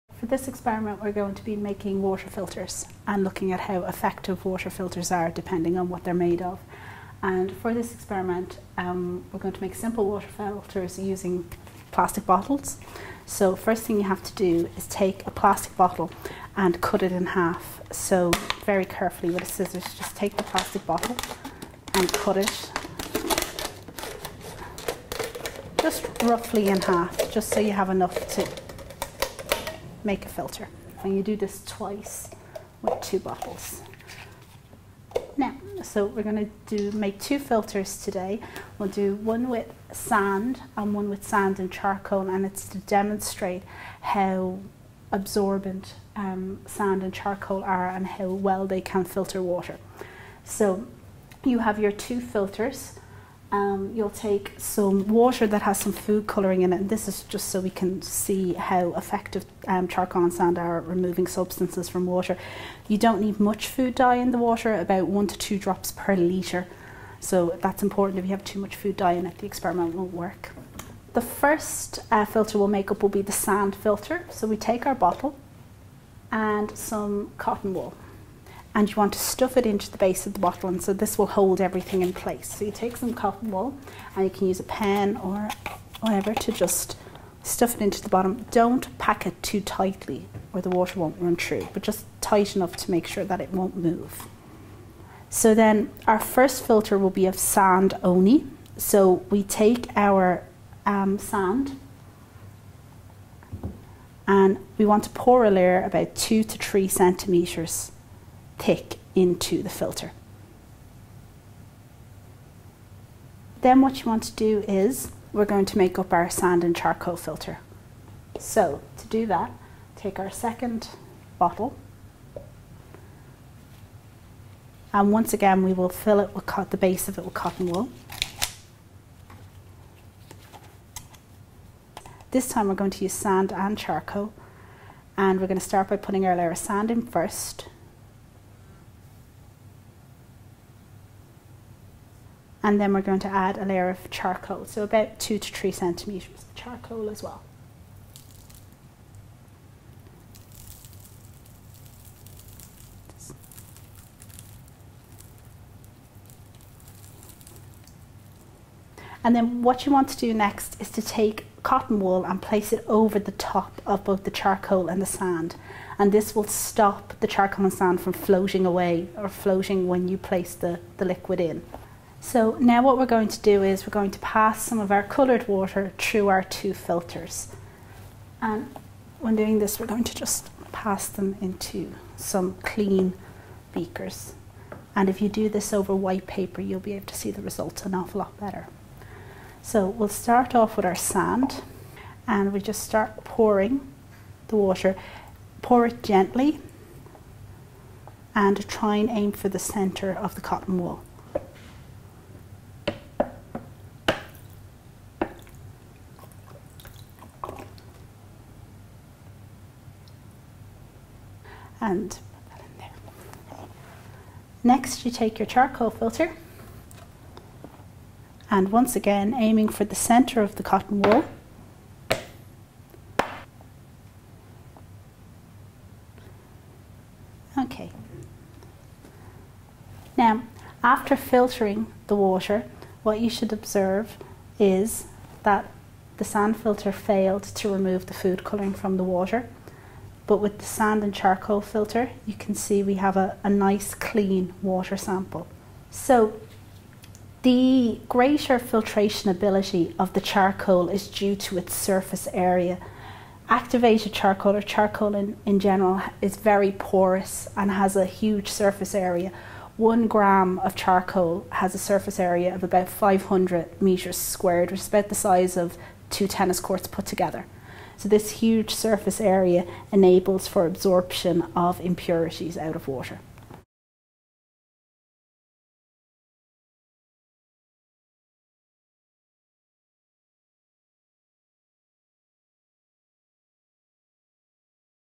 Environment - Experiment One
Investigate the filtering ability of Nano carbon (activated charcoal).